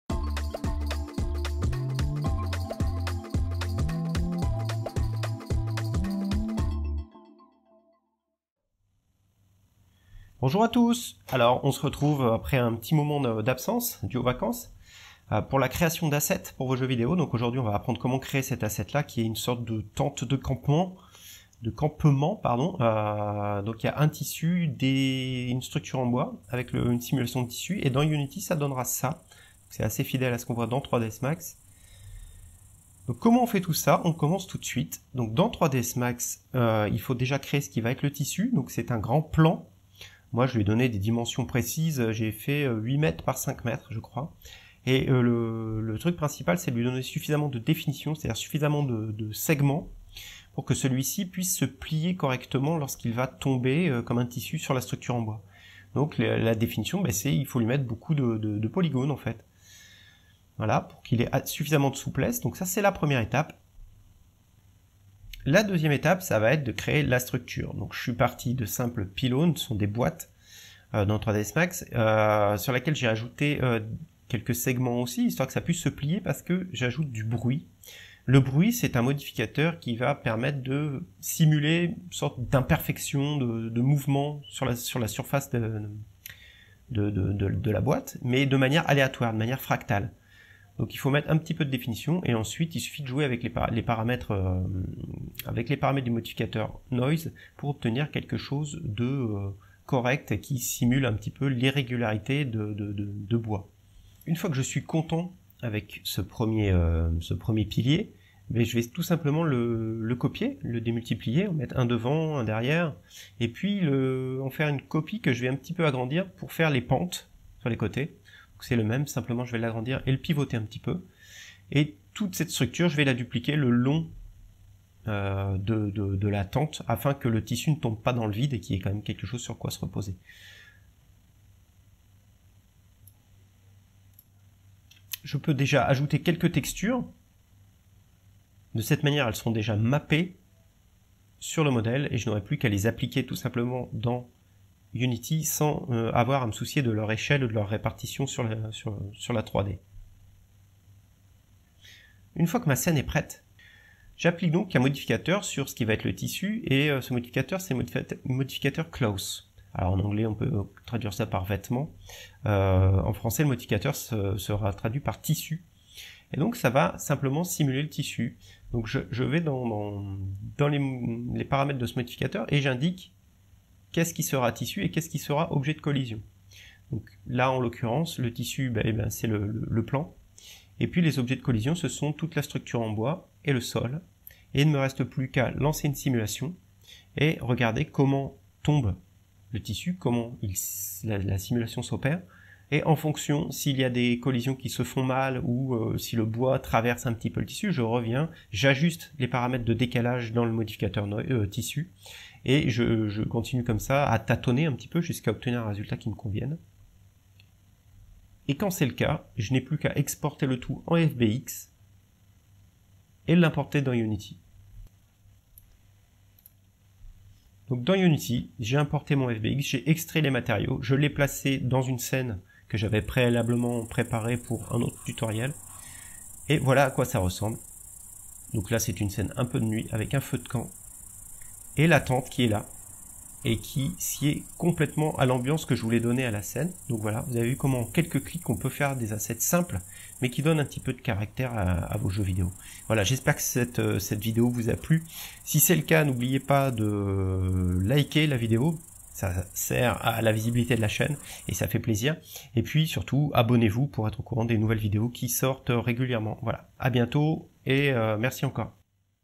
Tuto 3DS MAX / UNITY - modéliser une tente en 3D
Dans cette vidéo j'utilise 3DS MAx pour créer une asset 3D pour vos jeux vidéos, en l'occurence un campement c'est à dire une tente. Le but consiste à simuler le comportement du tissu en 3D avec le modificateur cloth de 3DS Max..

Suivez moi  !

LIKEZ si vous avez aimez !